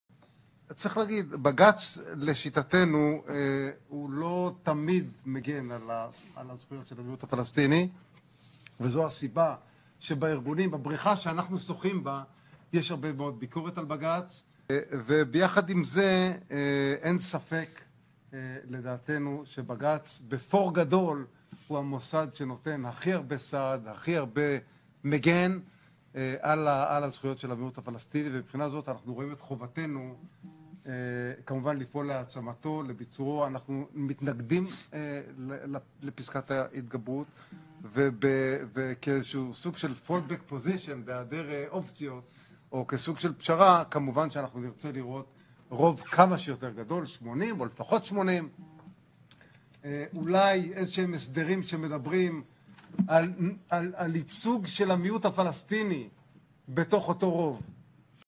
אמנון בארי-סוליציאנו על פסקת ההתגברות
אמנון בארי-סוליציאנו מתייחס לפסקת ההתגברות ופגיעתה הצפויה בהגנת זכויותיהם של אזרחי ישראל הפלסטינים, במפגש החשיבה "כיפת ברזל לדמקרטיה: הפגיעה בדמוקרטיה הישראלית, מערכת המשפט וזכויות אדם בישראל" שארגנו חברי הכנסת הרב גלעד קריב ועו"ד אפרת רייטן, משכן הכנסת, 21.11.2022.